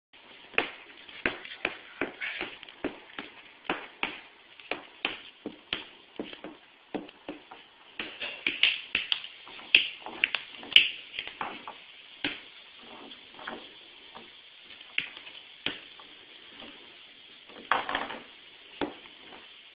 Tap-Pointe Shoes
cost about $38.00 not including shipping

My very first pair of tap-pointe shoes from Sansha! Sorry about Durango walking by in the beginning, he loves the camra! I'll be making more videos, so please check back soon! Ps Durango is a dog, he is a purebred keeshond